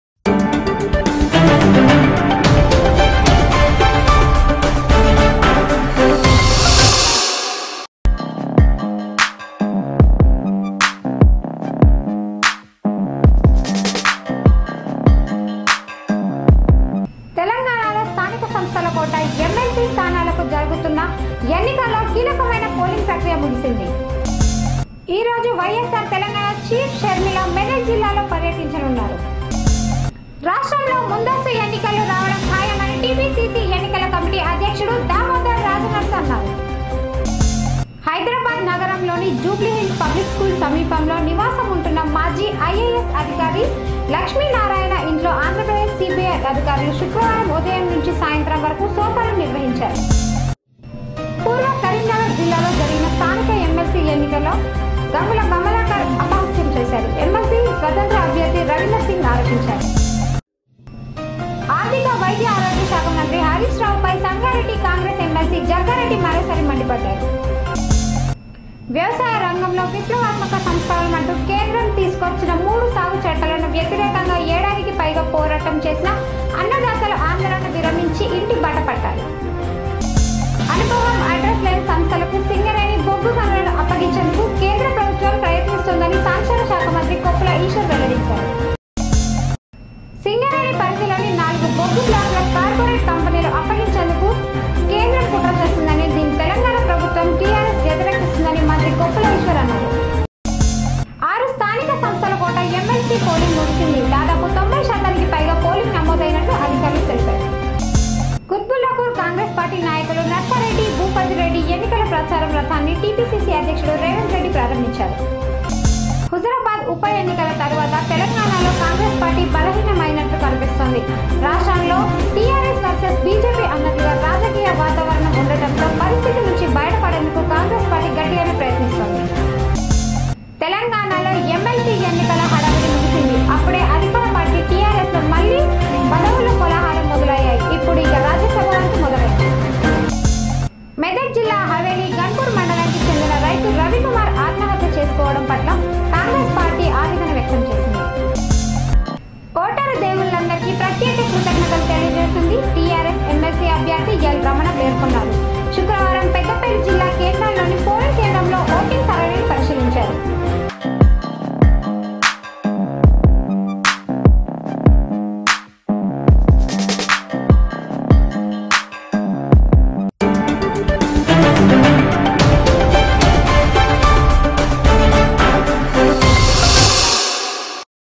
తెలంగాణలో స్థానిక సంస్థల కోటా ఎమ్మెల్సీ స్థానాల ఎన్నికల్లో కీలకమైన పోలింగ్ ప్రక్రియ ముగిసింది|C E N
Speed News|తెలంగాణలో స్థానిక సంస్థల కోటా ఎమ్మెల్సీ స్థానాలను జరుగుతున్న ఎన్నికల్లో కీలకమైన పోలింగ్ ప్రక్రియ ముగిసింది|Crazy E News.